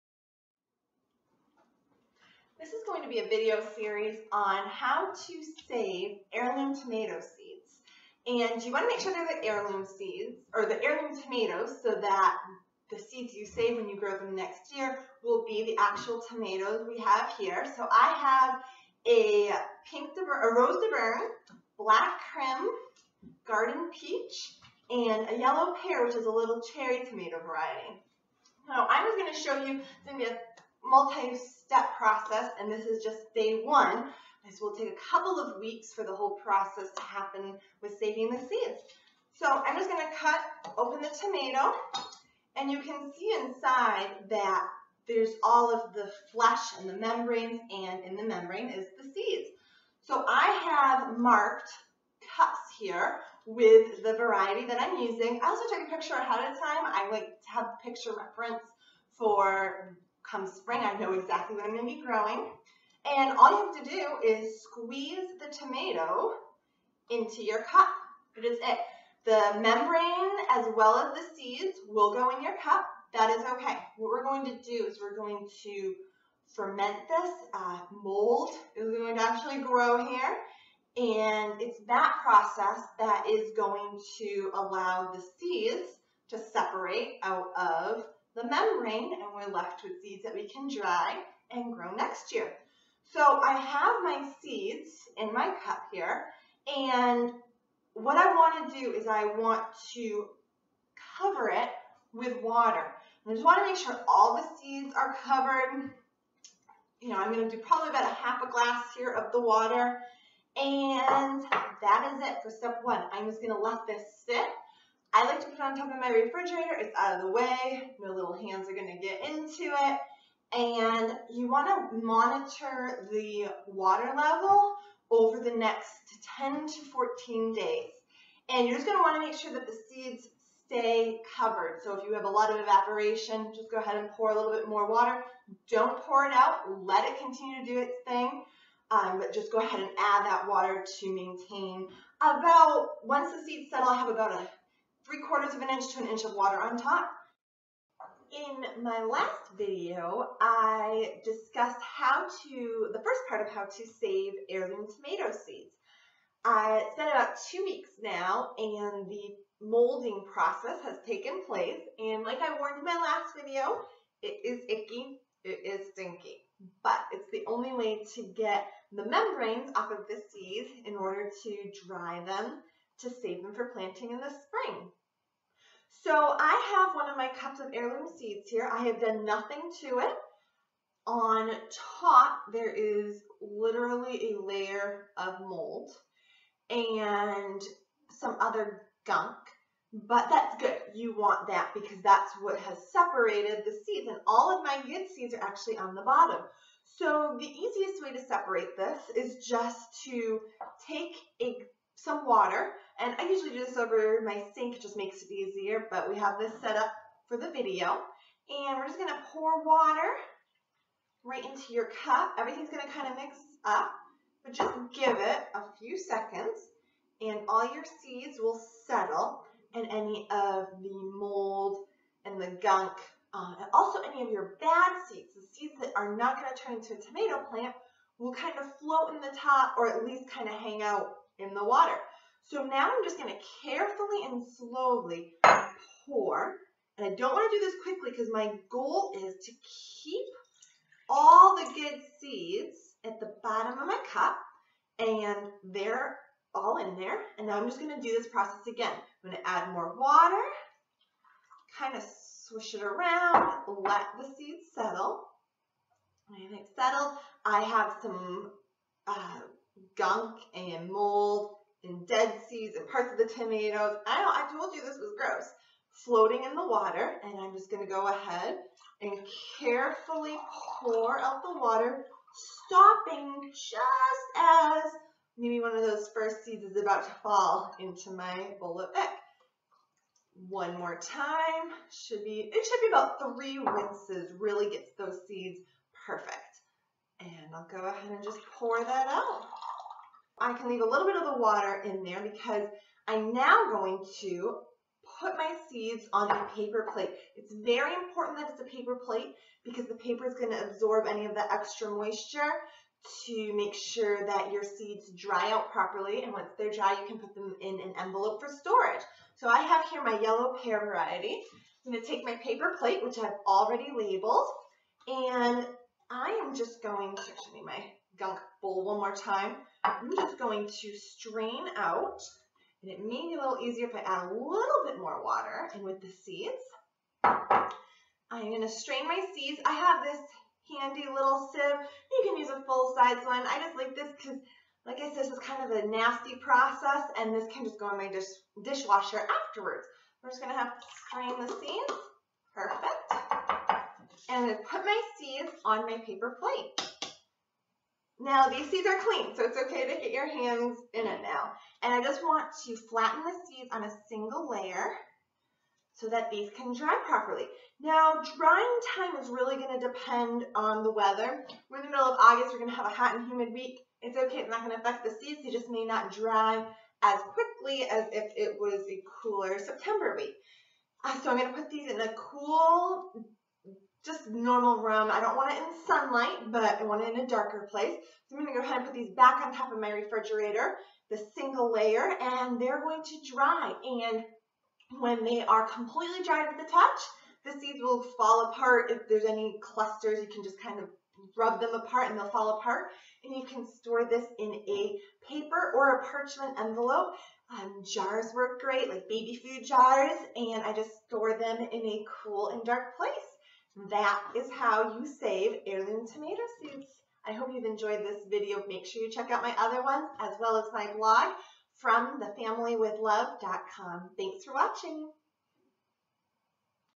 Heirloom Tomatoes: Step by step how to save heirloom tomato seeds (full process)
Shows step by step how to save heirloom tomato seeds.
-how to extract seeds
-how to ferment seeds to seperate from membrane
-how to wash seeds
-how to dry seeds
-tips for storing for next Spring's planting